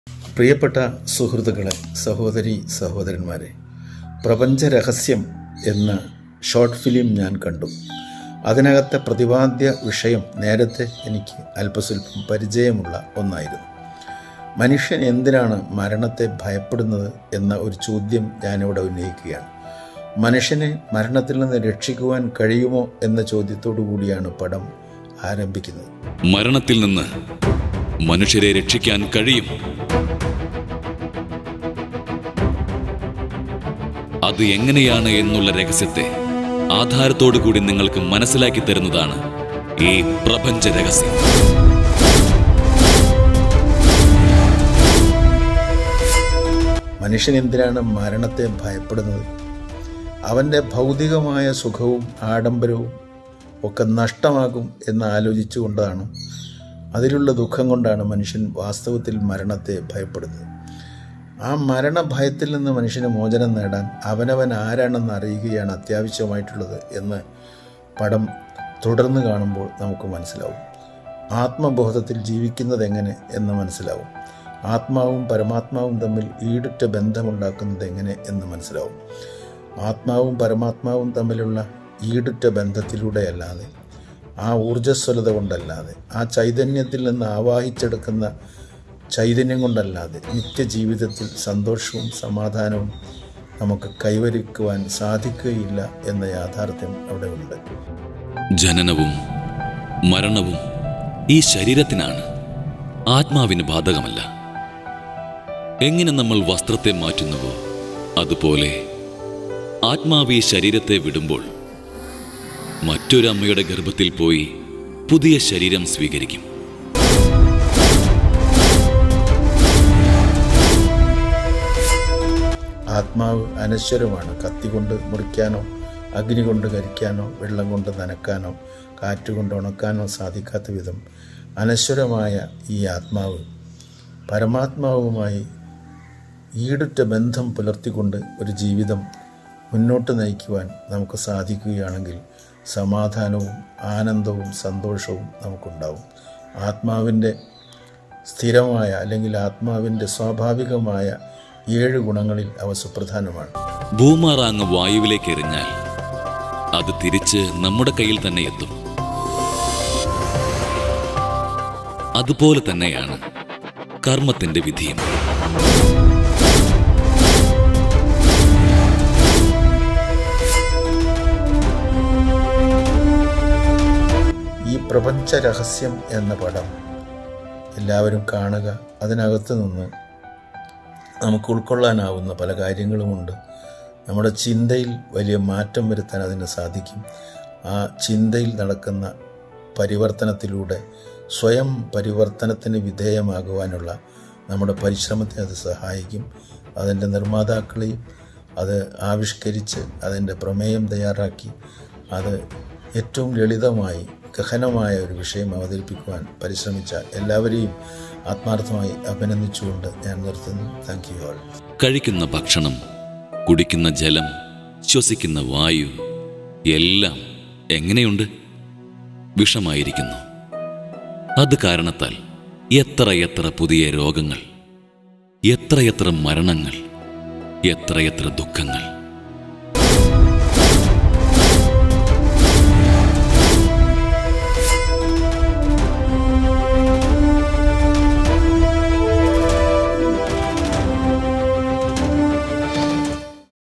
Adv. Sri K.N.A. Khader about ''Prapancha Rahasyam" Film | Brahmakumaris Keralam
PRAPANCHA RAHASYAM | Secrets of Universe | This Truth will Transform | New Knowledge for New World

മീഡിയകൾ കൈകാര്യം സഹോദരങ്ങളെ, കാലഘട്ടത്തിന്റെ ആവശ്യമായ ഈ അറിവുകൾ പൊതുജന സമക്ഷം എത്തിക്കുവാൻ ഈ വീഡിയോ പരമാവധി പ്രചരിപ്പിക്കുക. മാനവ സേവനത്തിനായുള്ള ഈ ഡോക്യൂമെൻ്റെറി മുഴുവനായോ ഭാഗങ്ങളായോ പ്രചരണാർത്ഥം ഉപയോഗിക്കുവാൻ വേണ്ടി വിളിക്കേണ്ട നമ്പർ - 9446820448